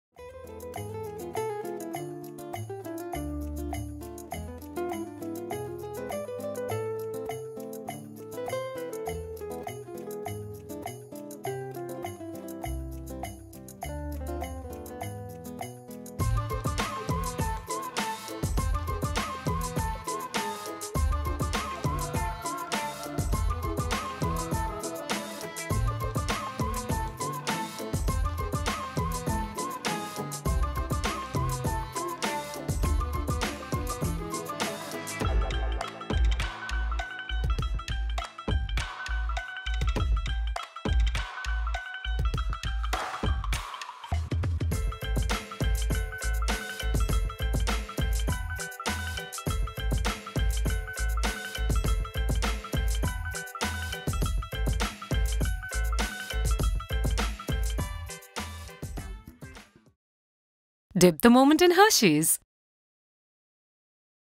Strawberry Baked Yoghurt Pudding | Chef Ranveer Brar Cook Book | AtHome with Hersheys
Ingredients
100 gm curd
100 gm cream
1 tbsp Custard Powder
30 ml HERSHEY’S Strawberry Flavoured Syrup

Process:
● Mix all the ingredients together
● Pour in ramekins till ¾
● Bake at 150 degrees for 10 minutes in a water bath
● Chill once it reaches room temperature
● Serve chilled